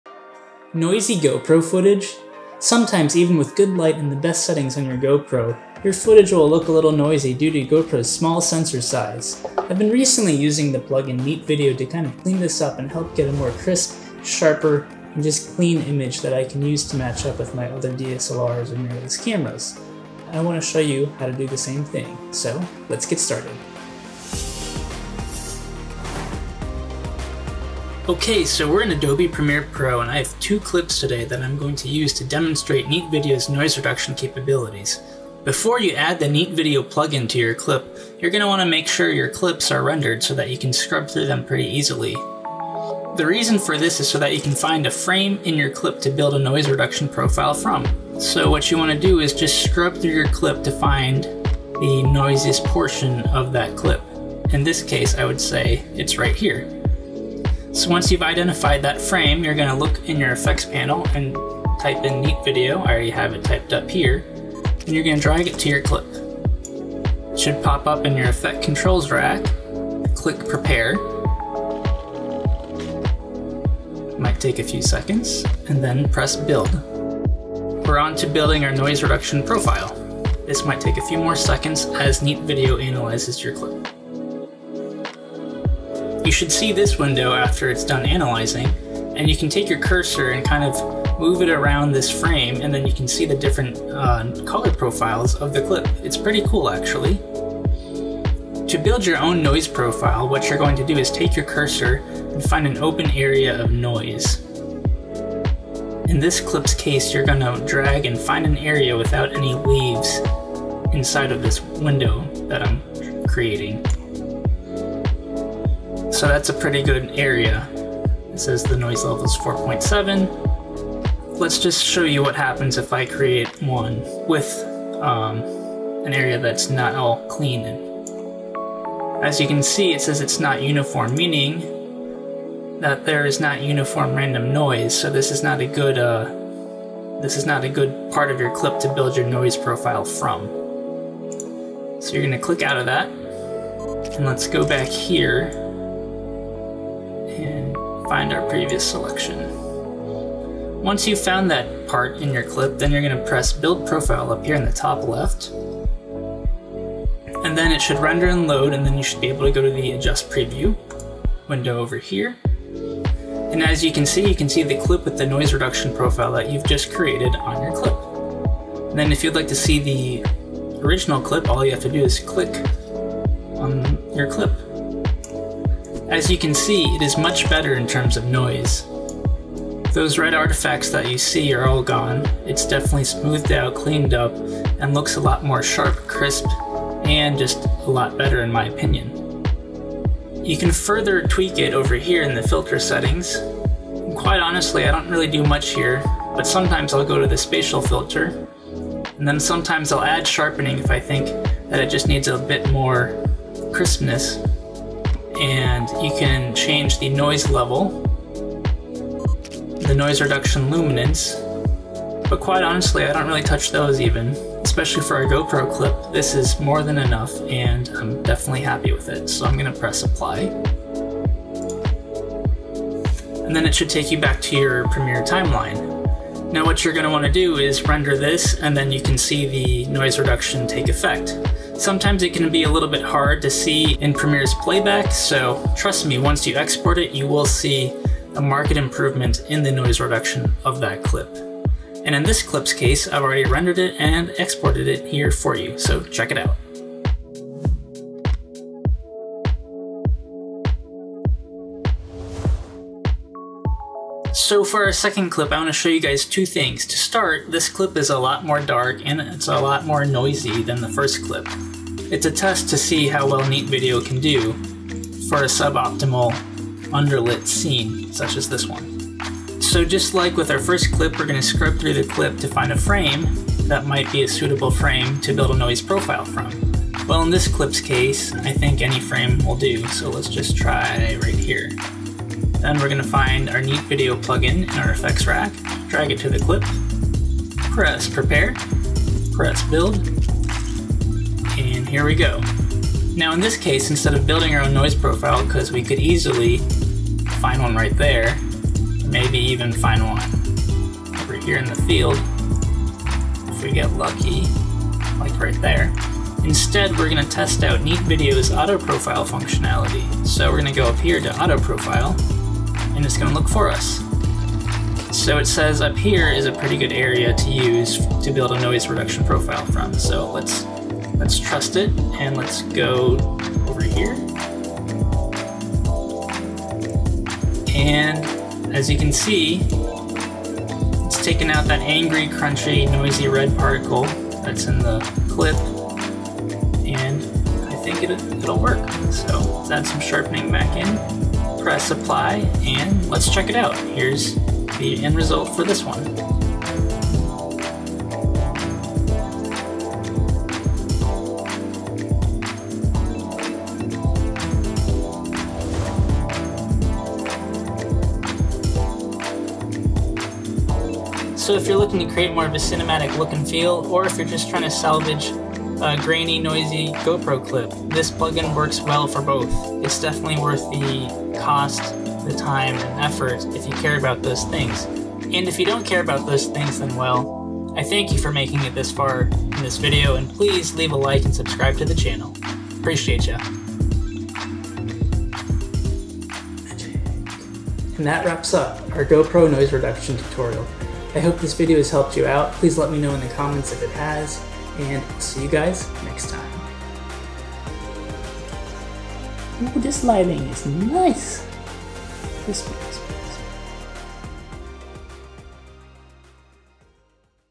Get Better Looking GoPro Footage [Remove Noise using Neat Video]
Get better looking GoPro footage, remove noise using the plugin, Neat Video. Sometimes even with decent lighting and proper settings, your GoPro video just looks a little noisy. Neat Video is a handy plugin that reduces video noise and can dramatically improve the quality and look of your footage.

Why Use Neat Video's Plugin - 0:00​
Neat Video Tutorial, Creating a Noise Profile - 0:27
GoPro Noise Reduction, Before And After 1 - 4:07
Can Neat Video Make An Unusable Clip, Usable? - 4:14
GoPro Noise Reduction Before And After 2 - 5:47
More Examples - 6:00

Camera 📷
-Go Pro Hero 9
--Fujifilm X-T4

Lens 📸
-Sigma 16mm F1.4 Contemporary DC DN Lens